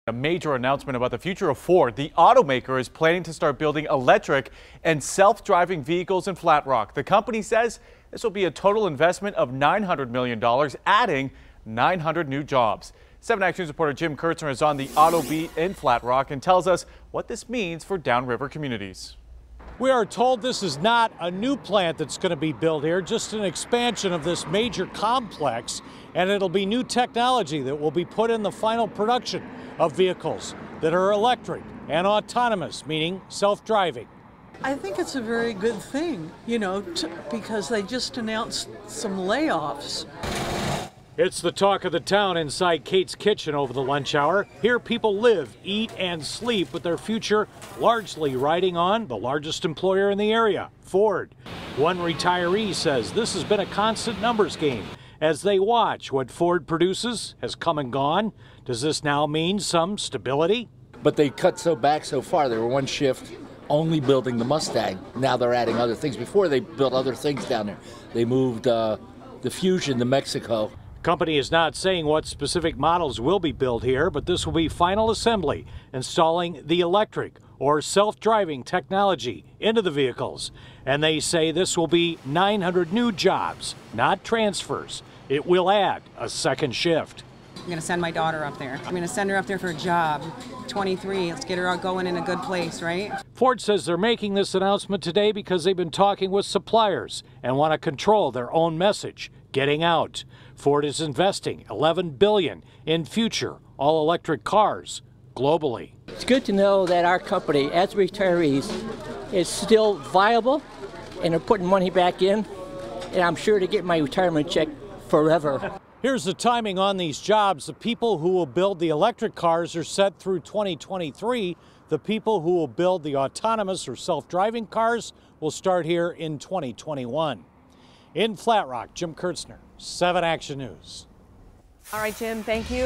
Ford to expand production at Flat Rock Assembly plant, will add 900 jobs in Michigan
Ford Motor Co. is repackaging a previously announced manufacturing investment in the Detroit area and now says it will spend $900 million and create 900 new jobs over the next four years.  Most of the new workers will build a new generation of electric vehicle at Ford's existing factory in Flat Rock, Michigan, south of Detroit, which will see an $850 million investment. The company also plans a roughly $50 million autonomous vehicle manufacturing center at an undisclosed site near Detroit that will add hardware to existing vehicles.